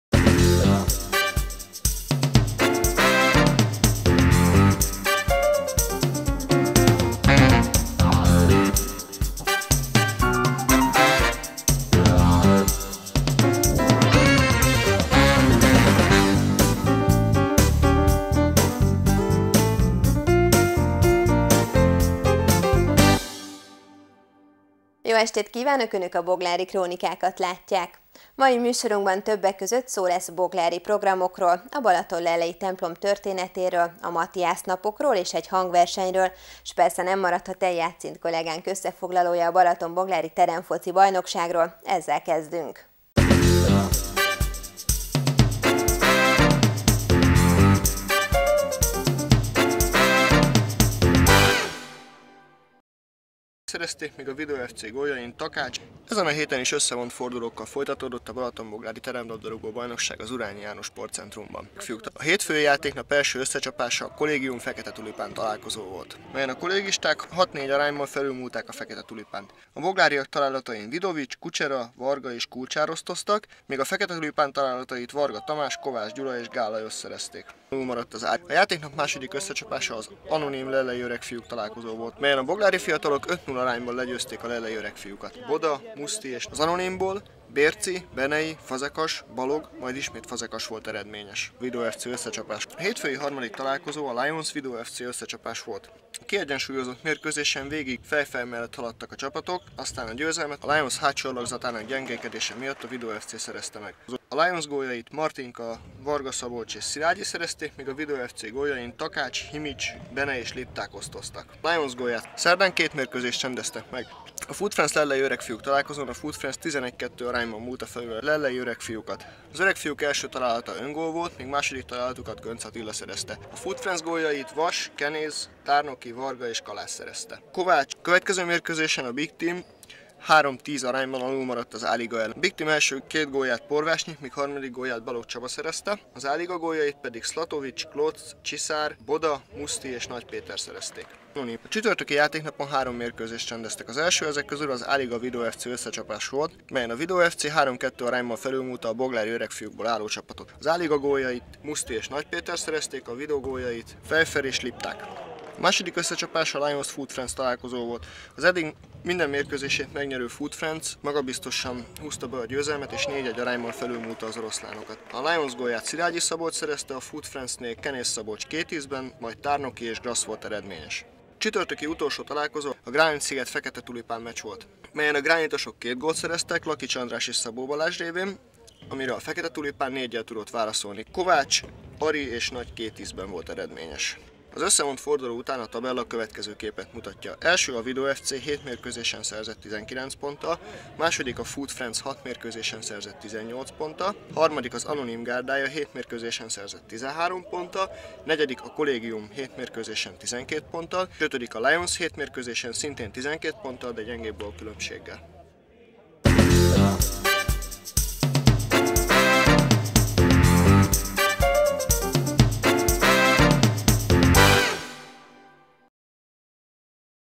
Balatonboglári Terembajnokság 10/11 7. ford.
Az Urányi sportcentrumban zajló Balatonboglári Teremlabdarúgó bajnokság hetedik fordulójának összefoglalója.
A felvétel a Supra tv/Boglár tv engedélyével került feltöltésre. Technikai okok miatt a szokásosnál rövidebb lett, de nem lesz ez mindíg így..a következő rész újra a megszokott 10 perc körüli hosszúságú lesz.